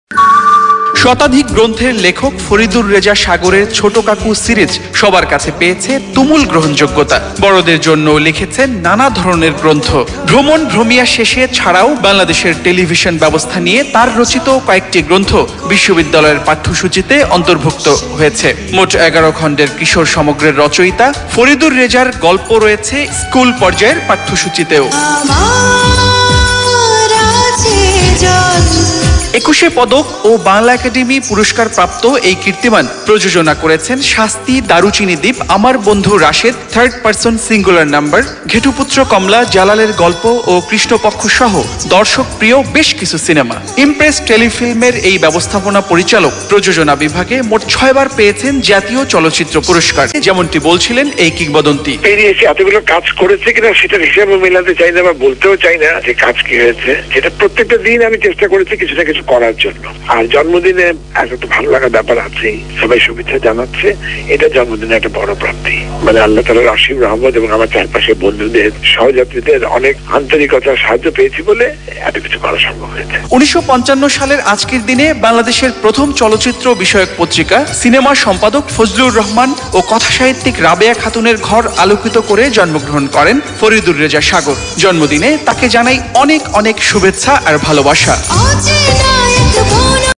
Faridyr Reza Sagor Bangla Art Film Concept
Faridur Reza Sagar was born on February 22, 1955 in Dhaka, Bangladesh. The eldest Son of famous Bangladeshi fiction writer Begum Rabeya Khatun and Director Fazlul Haque. Since childhood, he has been involved with many children-friendly TV Shows on Bangladesh Television. In 1966, Faridur Reza Sagar himself played the leading role in the very first children feature film 'President' directed by his father. He earned fame as a young presenter, program conductor and involving with television program since the inception of Bangladesh Television. For over 20 years, he has been writing fictions for the children and more than 50 books have been published. He has also written a number of scripts and television dramas for the children.

He is the Managing Director of Channel I, the first digital television channel in Bangladesh launched on October 1, 1999 which broadcasts over 89 countries around the world and the most popular television channel of the country. He is also at the Board of Directors of Impress Group, one of the largest groups of companies in Bangladesh.

In 2005, for his very special contribution for Children Literature he was honored with the Bangla Academy Award. In addition, he also received Agrani Bank Children's Literature Award, Chader Haat Children's Literature award, Euro Children's Literature Award. He was also awarded with Bangladesh Film Journalists' Association Award, Cultural Reporters' Award, National Film Award (2002) and many other National Awards.

He produced over 42 full-length feature films, which earned numerous International Nominations and Awards.

Faridur Reza Sagar is a living legend. With wife Kona Reza and two daughters Meghna and Mohona, he has a complete cultural atmosphere at home.